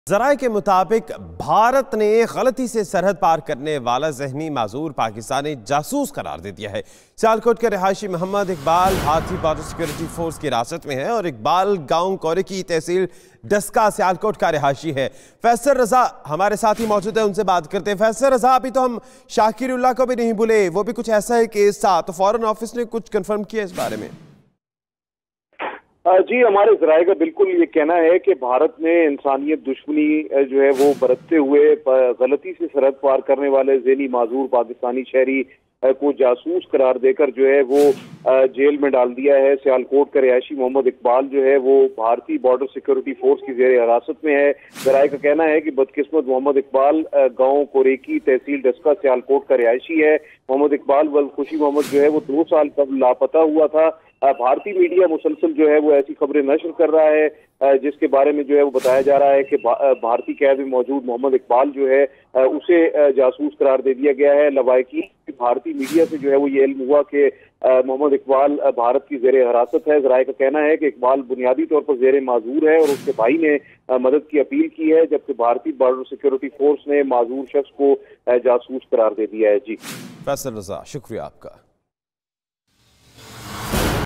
Mentally disabled man accidentally crossed border, India declares him a Pakistani Spy
Mentally disabled man accidentally crossed border, India declares him a Pakistani Spy | 24 March 2019 | 92NewsHD